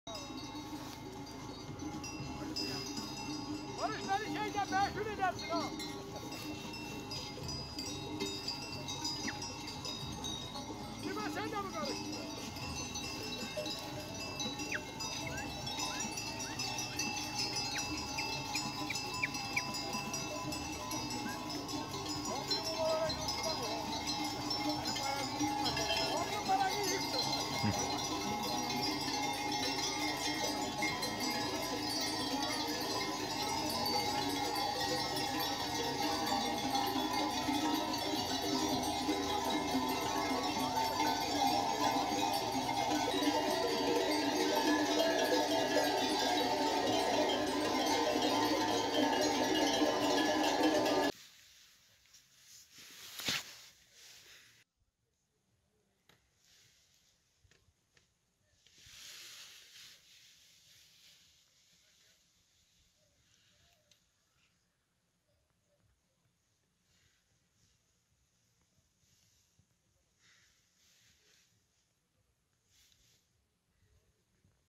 Ordu'ya mevsimin ilk karı yağdı
Ordu'nun Kabadüz ilçesinde yağan mevsimin ilk karıyla Çambaşı Yaylası beyaza büründü